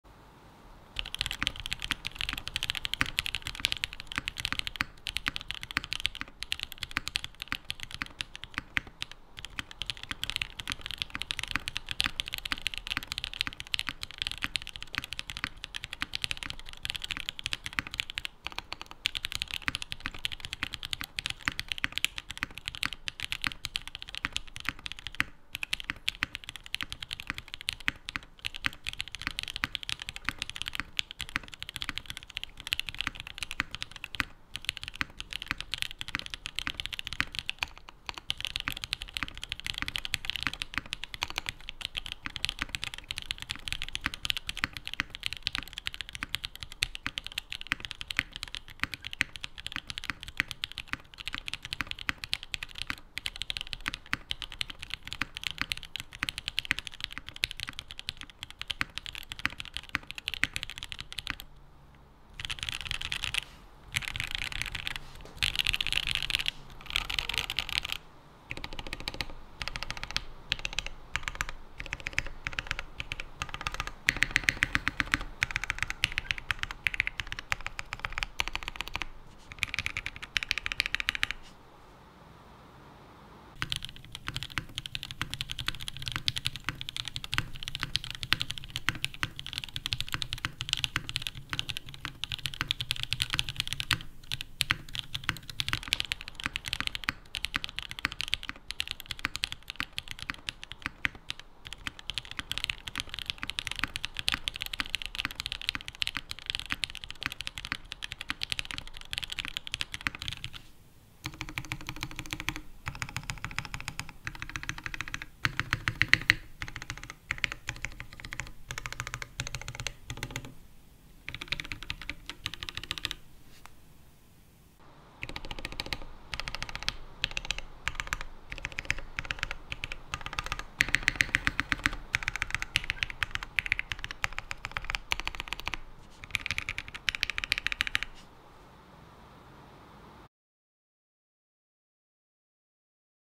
Hiney TKL One. No mod vs Tape mod at the end. Typing sound: Tape mod on
Tape mod vs no mod at 1:23
I apply masking tape on the bottom of pcb with 1-2 layers of the tape
credit to oTEMPE5To for the tape mod!

Case: Gold Hiney TKL One
Mounting: burger top mount
Foam: no foam
Plate: Polycarbonate
Switch: SP-Star Polaris white (linear) lubed with 204g0
Spring: stock with 105g0
Film: tx film
Stabilizer: Durock v2 lubed with 205g0 and permatex
Keycap: GMK Ursa
Deskmat: none

Please excuse the misplace between shift and caplocks lmao

recorded using A7riii + Rode Videomic NTG shotgun mic
unedited sound